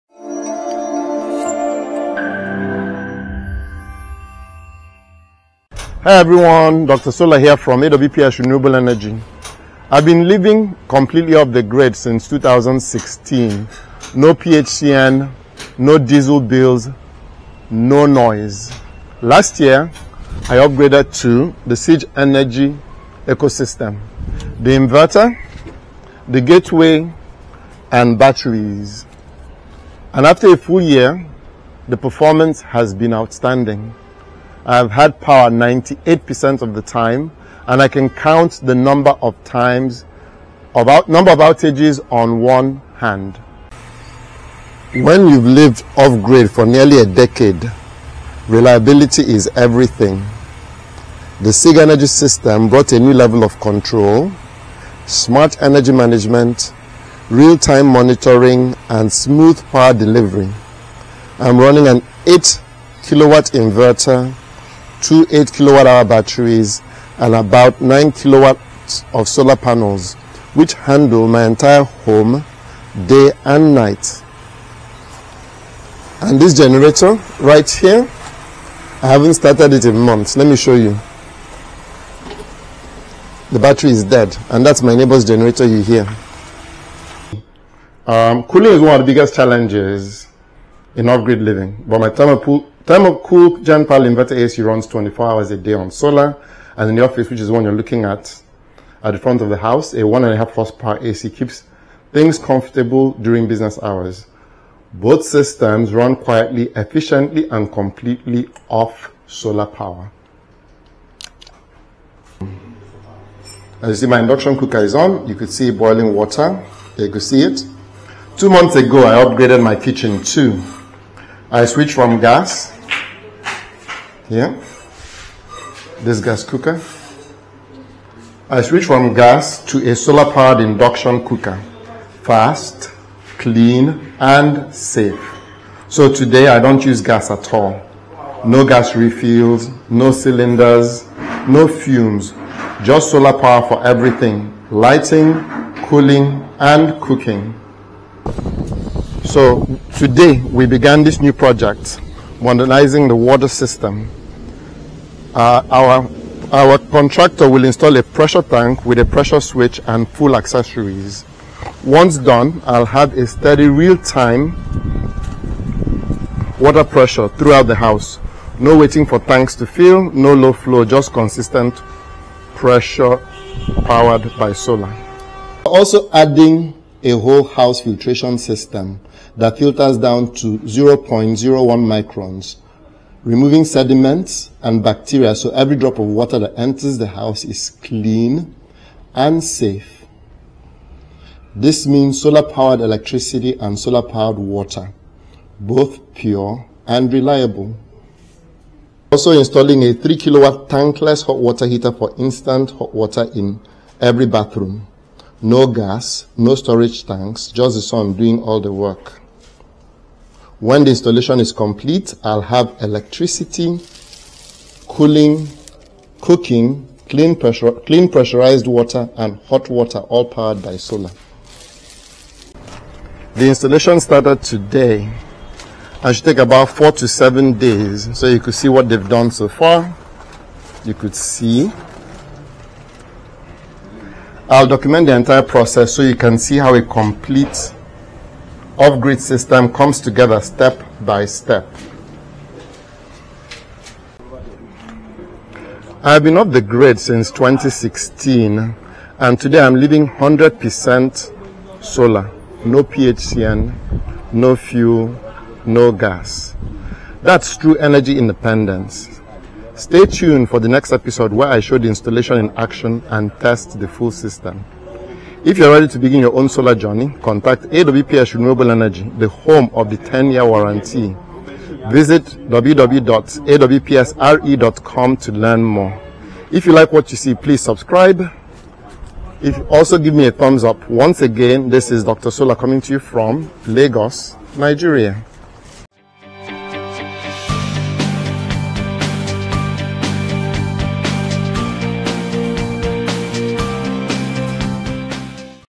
🔋 “9 Years Off-Grid in Nigeria — The Complete Solar Home”
I’ve been living completely off the grid since 2016 — no PHCN, no fuel, no noise.
Last year, I upgraded to the SigEnergy inverter, gateway, and battery system, and after one full year, the performance has been nothing short of amazing.

In this video, I share how I’ve built a fully solar-powered home here in Nigeria — where everything runs on the sun:
☀️ 8 kW SigEnergy inverter + 16 kWh battery bank
⚡ 9 kW of solar panels
❄️ Thermocool Genpal inverter A/C (24 hours/day)
🏢 Office 1.5 HP inverter A/C
🍳 Induction cooker — no gas, no refills
💧 Upcoming pressure-tank and whole-house filtration system
♨️ 3 kW tankless hot-water heater installation starts tomorrow

For 9 years I’ve lived without the grid, and this new phase takes it even further — upgrading water pressure, filtration, and hot water — all powered by solar energy.

If you’ve ever wondered whether it’s possible to live completely off-grid in Nigeria, this video proves it’s not only possible — it’s comfortable.